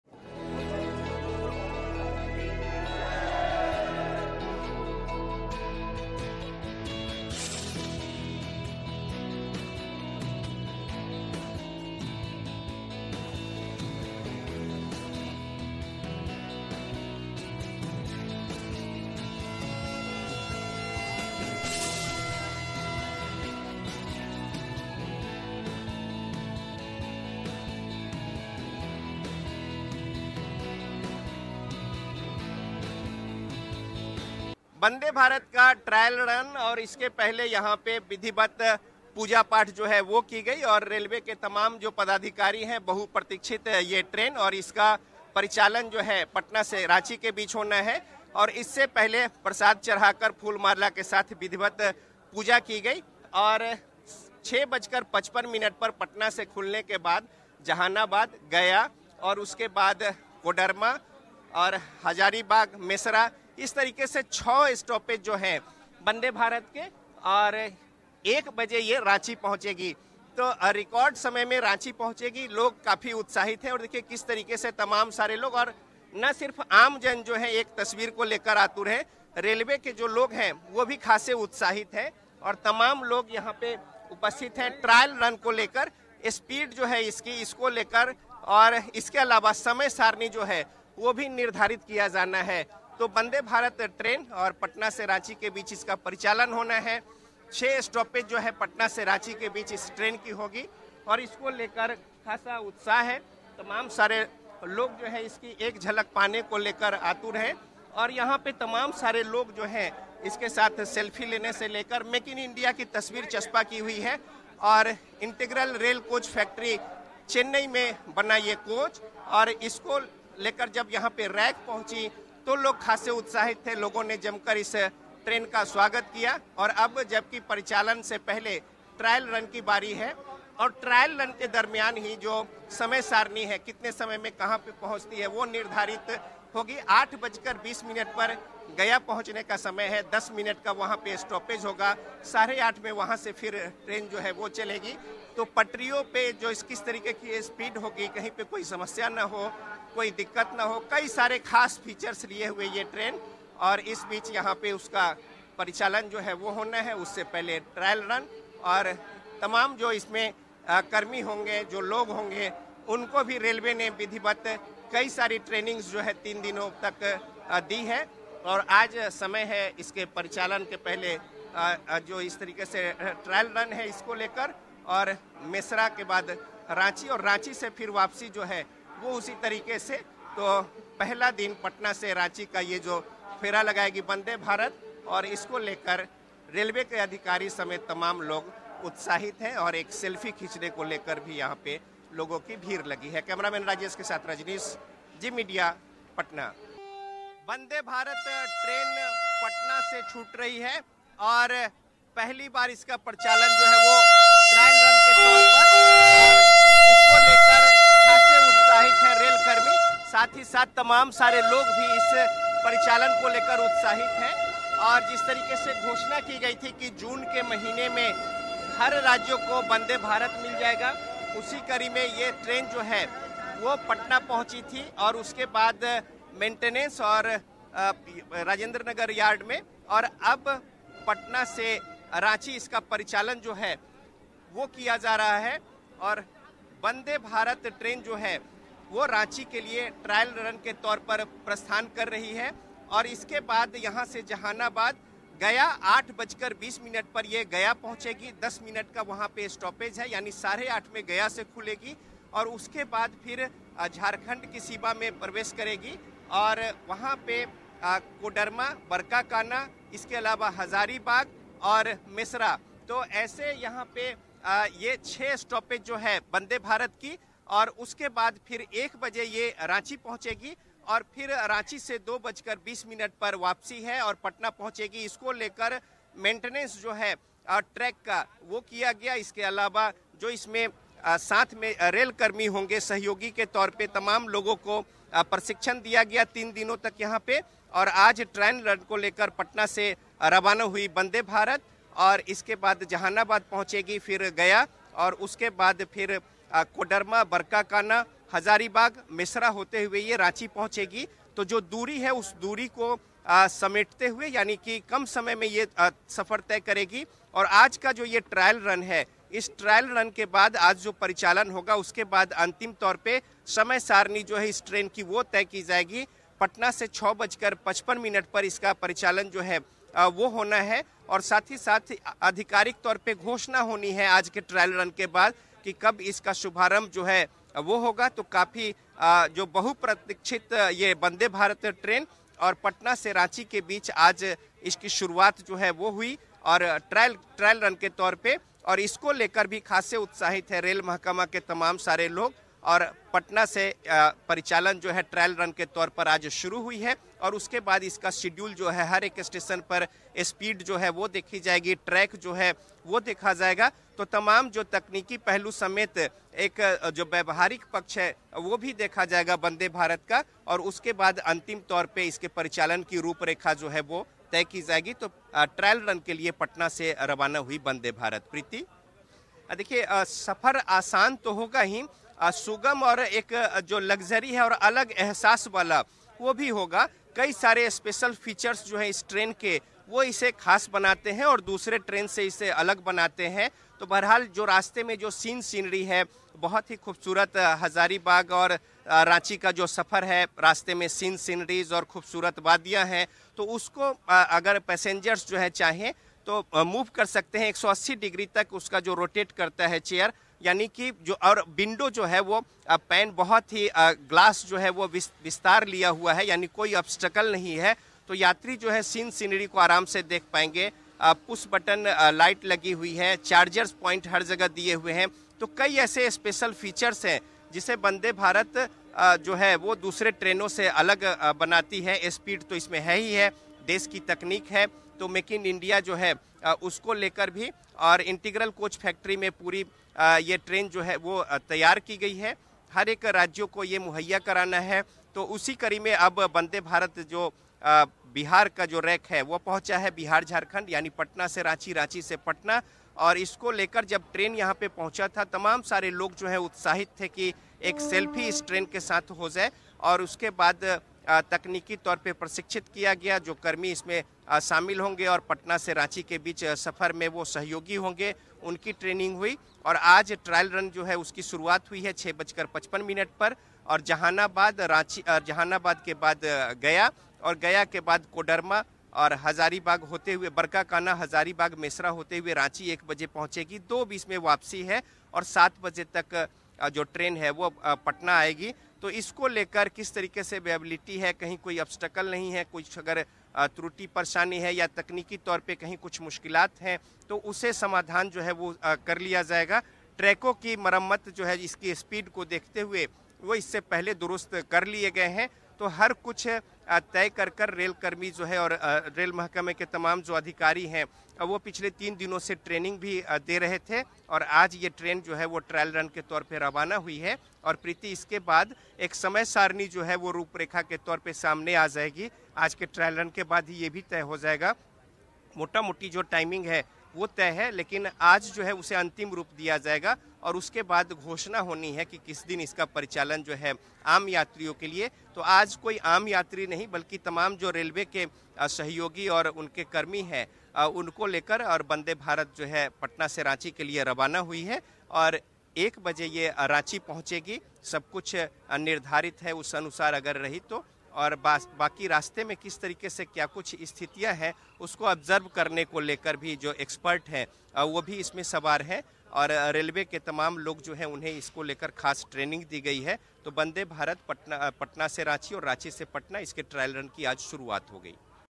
Vande Bharat Express: रांची और पटना के लोगों का सपना हुआ पूरा | Patna | Ranchi | Zee Bihar Jharkhand
Vande Bharat Express: पटना और रांची के बीच आज वंदे भारत एक्सप्रेस (Patna Ranchi Vande Bharat Express) का ट्रायल रन (Vande Bharat train Trial run) शुरू हुआ है...जिसे देखने के लिए रेलवे स्टेशन पर पहुंचे लोगों में भारी उत्साह नजर आया...वंदे भारत एक्सप्रेस (Vande Bharat Express) ट्रेन पटना जंक्शन (Patna Junction) से सुबह 6:55 बजे खुलकर बरकाकाना होते हुए दोपहर 12:47 बजे रांची पहुंची...इस दौरान सभी स्टेशन पर वंदे भारत को देखने के लिए लोगों की भीड़ लगी रही.